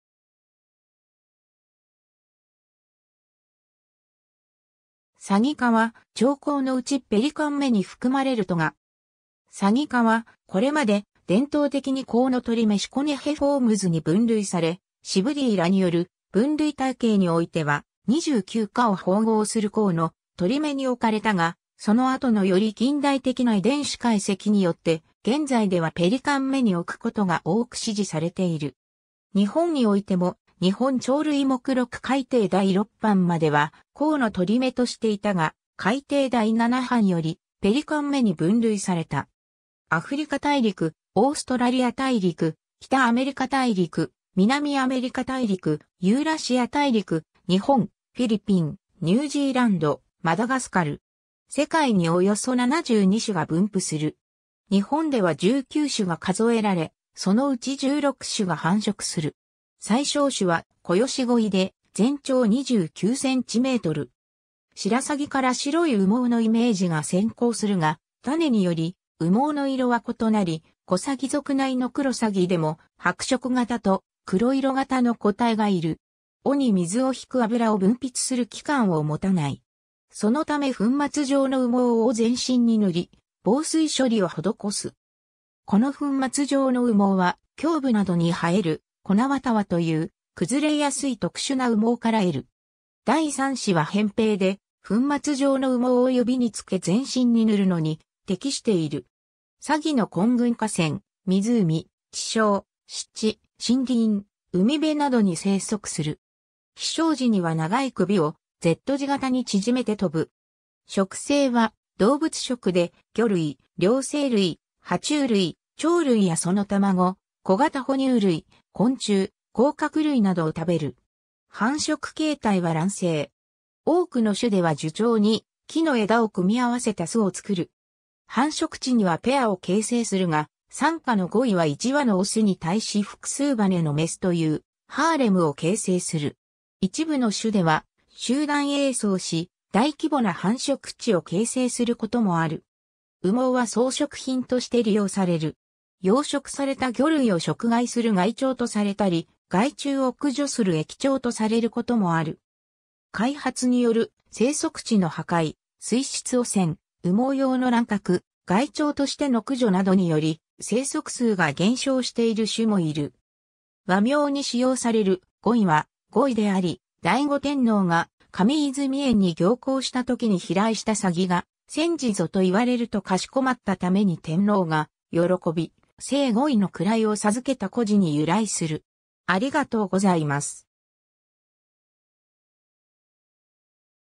サギ科
サギ科（サギか、Ardeidae）は、鳥綱のうちペリカン目に含まれる科。
 サギ科は、これまで伝統的にコウノトリ目 Ciconiiformes に分類され、Sibley らによる分類体系 (1990) においては、29科を抱合するコウノトリ目に置かれたが、その後のより近代的な遺伝子解析によって、現在ではペリカン目に置くことが多く支持されている。
日本においても、日本鳥類目録改訂第6版 (2000) まではコウノトリ目としていたが、改訂第7版 (2012) よりペリカン目に分類された。
 アフリカ大陸、オーストラリア大陸、北アメリカ大陸、南アメリカ大陸、ユーラシア大陸、日本、フィリピン、ニュージーランド、マダガスカル。
 世界におよそ72種が分布する。
日本では19種が数えられ、そのうち16種が繁殖する。
 最小種はコヨシゴイで全長29センチメートル。
白鷺から白い羽毛のイメージが先行するが、種により羽毛の色は異なり、コサギ属（旧・シラサギ属）内のクロサギでも白色型（全身の羽毛が白い）と黒色型（全身の羽毛が黒い）の個体がいる。
 尾に水を弾く脂を分泌する器官（羽脂腺）を持たない。
そのため粉末状の羽毛を全身に塗り、防水処理を施す。
この粉末状の羽毛は、胸部などに生える粉綿羽という崩れやすい特殊な羽毛から得る。
第3指は扁平で、粉末状の羽毛を指につけ全身に塗るのに適している。
 サギの混群 河川、湖、池沼、湿地、森林、海辺などに生息する。
飛翔時には長い頸をＺ字型に縮めて飛ぶ（この飛翔姿勢で、コウノトリ目やツル目の類似種と区別できる。ペリカン目の中ではペリカン科とハシビロコウ科が同じように頸を縮めて飛ぶが、シュモクドリ科とトキ科は頸を伸ばして飛ぶ）。
 食性は動物食で、魚類、両生類、爬虫類、鳥類やその卵、小型哺乳類、昆虫、甲殻類などを食べる。
多くの種では樹上に木の枝を組み合わせた巣を作る。
繁殖地にはペアを形成するが、サンカノゴイは1羽のオスに対し複数羽のメスというハーレムを形成する。
一部の種では集団営巣し、大規模な繁殖地（コロニー）を形成することもある。
 羽毛は装飾品として利用される。
養殖された魚類を食害する害鳥とされたり、害虫を駆除する益鳥とされることもある。
開発による生息地の破壊、水質汚染、羽毛用の乱獲、害鳥としての駆除などにより生息数が減少している種もいる。
 和名に使用される「ゴイ」は「五位」であり、醍醐天皇が神泉苑に行幸したときに飛来したサギが「宣旨ぞ」と言われるとかしこまったために天皇が喜び「正五位」の位を授けた故事に由来する。